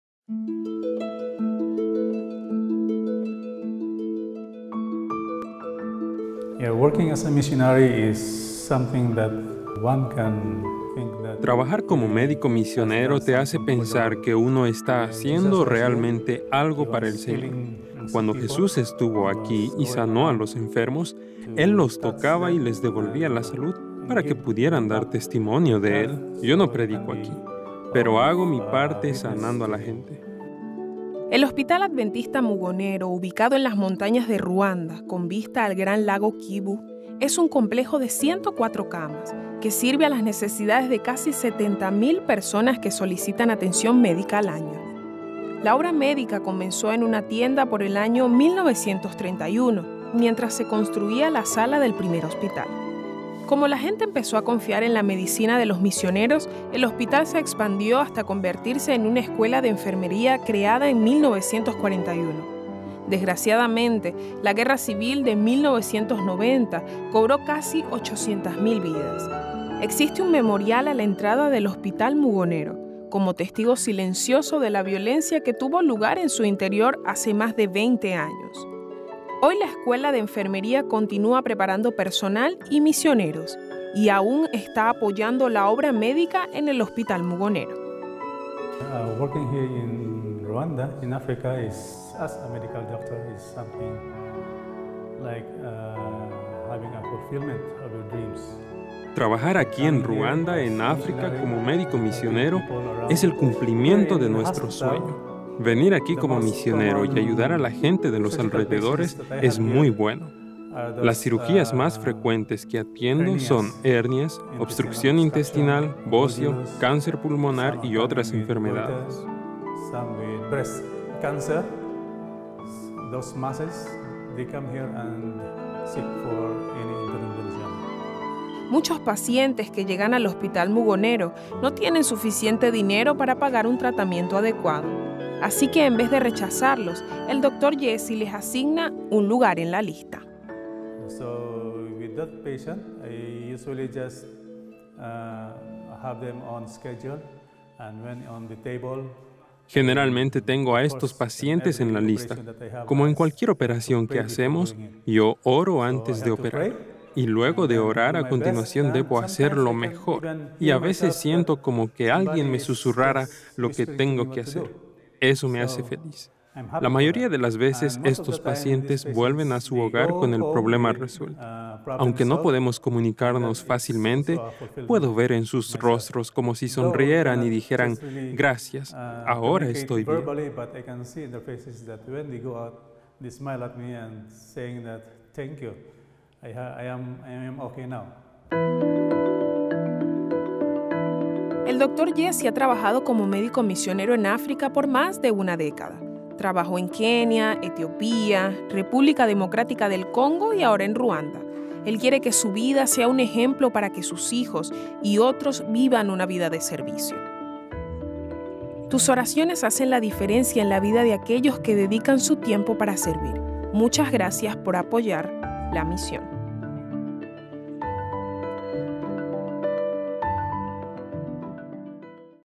Hospital Mugonero (Ruanda)
Al venir como misionero no puedes esperar una gran recompensa económica, no contamos con el dinero que recibimos de vez en cuando, porque sabemos que hay algo mejor preparado para nosotros si trabajamos para Jesús,